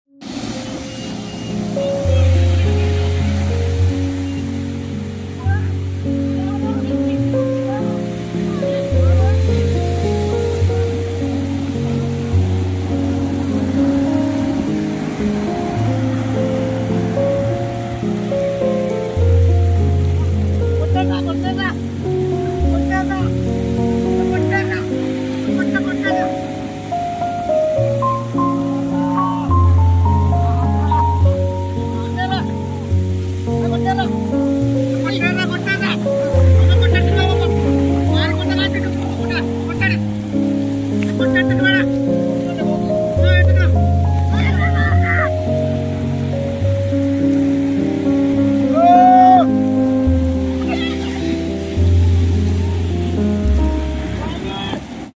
FISH WASHED UP ON THE BAPATLA. BEACH. బాపట్ల సముద్ర తీరానికి కొట్టుకువచ్చిన చేపలు
the cyclone effect